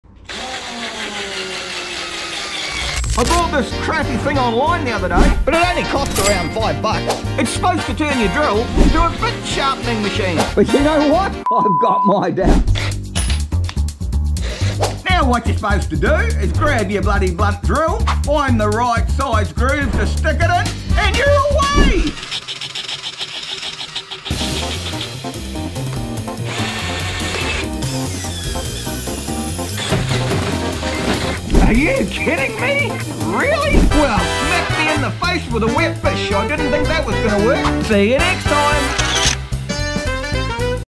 Mind-Blowing Drill Bit Sharpening Hack
Learn surprising budget drill bit sharpening tricks in this DIY video. Discover how to sharpen drill bits with simple tools and save money on replacements.

Hey there! Let's keep the DIY fun going strong by buying me a coffee – your support helps fuel our creative adventures together!